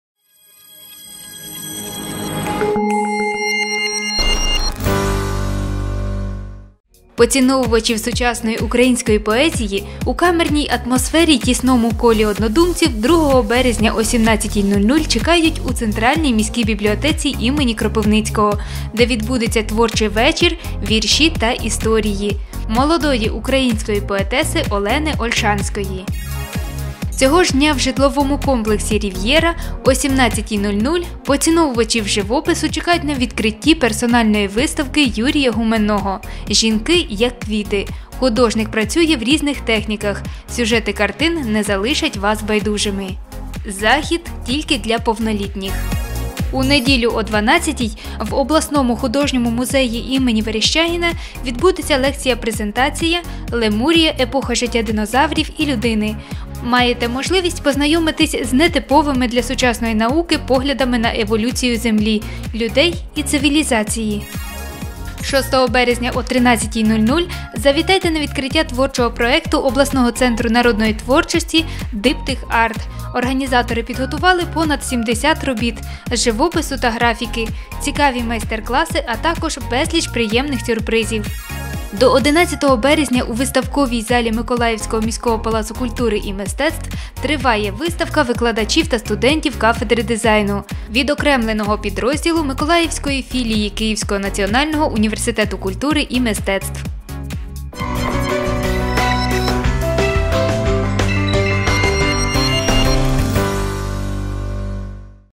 Новий день. Афіша
Погода за вікном вас не лякає? Тоді для вас добірка культурних заходів від наших журналістів, які відбуватимуться 1-2 березня. Вибирайте собі розваги по душі.